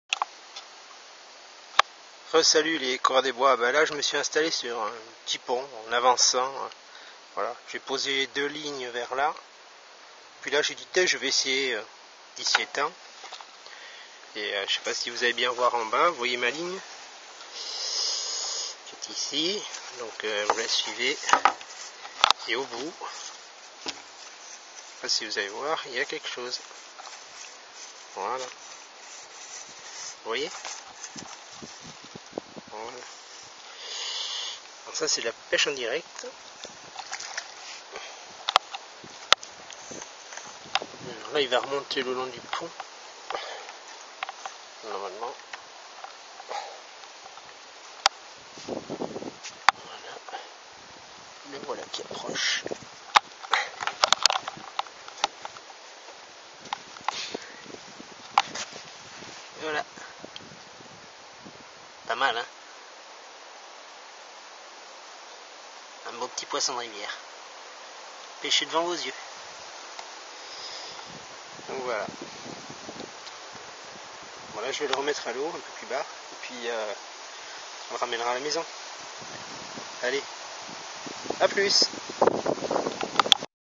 Suite de la partie de pêche
La partie de pêche se poursuit avec une capture en direct .